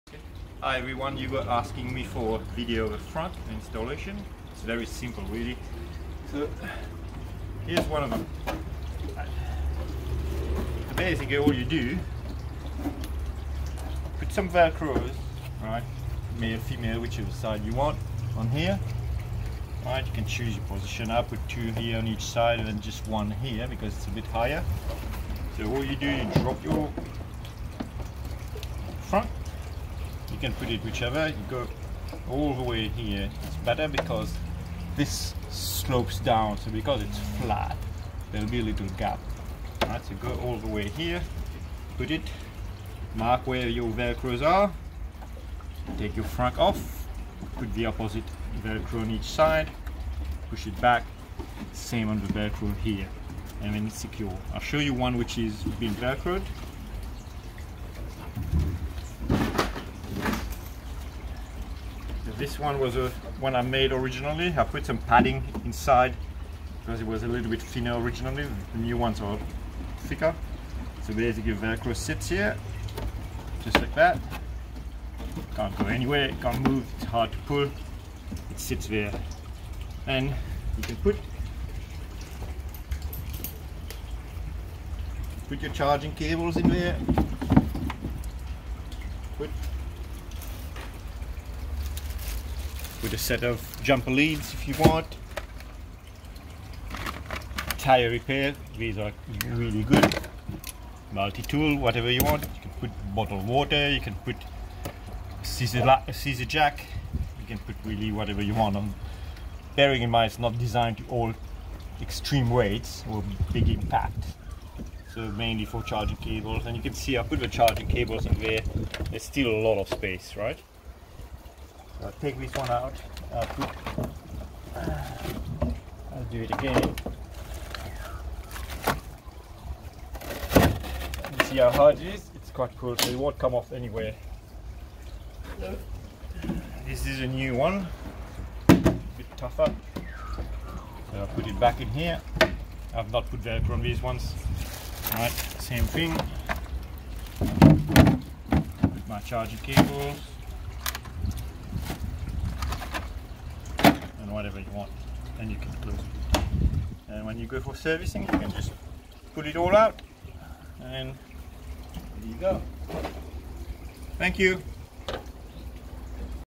byd atto 3 frunk
this is a removable frunk for the byd atto 3  for australia  RHD version
allows for air flow around components . allow access to the battery.   maximise space.  easily removed when car goes for service.